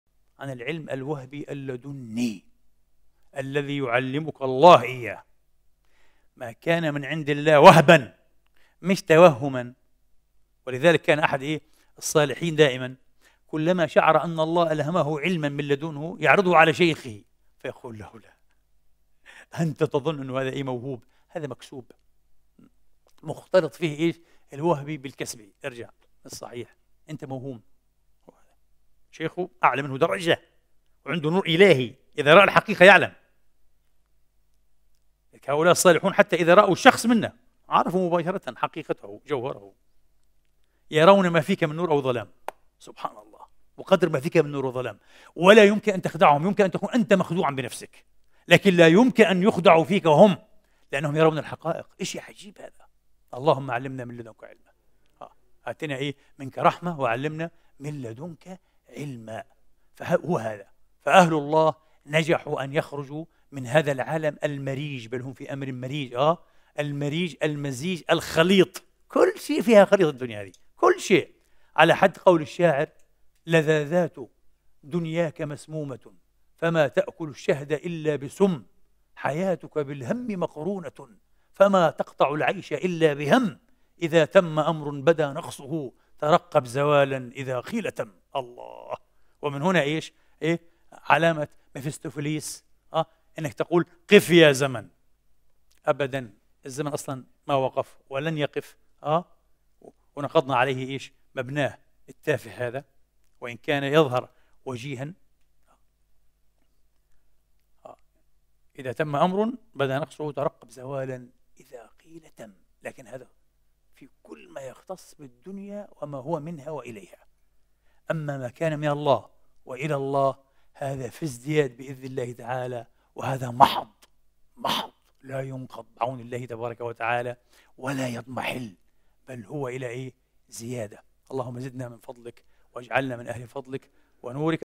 🔥 هل تعلم أنه يعرف حقيقتك، ويعرف ما فيك من نور وظلم؛ لأنه ولي لله؟ عدنان إبراهيم، الكلاب البشرية
من الكلاب البشرية للدكتور عدنان إبراهيم

للوصول إلى حسابات فضيلته الرسمية:
adnanibrahim#
adnan_ibrahim#